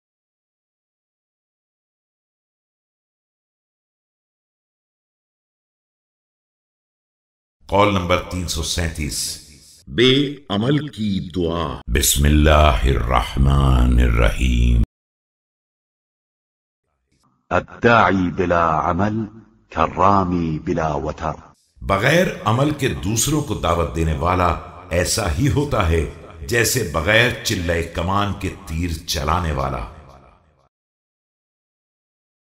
Nahjul Balagha | Qaul 337 with Urdu translation by Allama Zeeshan Haider Jawadi
نہج البلاغہ قول نمبر 337 عربی تلاوت، اُردو ترجمہ کے ساتھ
 اردو ترجمہ : علامہ ذیشان حیدر جوادی
 عربی تلاوت : قاری جواد رفیعی
 اردو آواز : شاہ رُخ مرزا

LIKE | COMMENT | SHARE | SUBSCRIBEvvv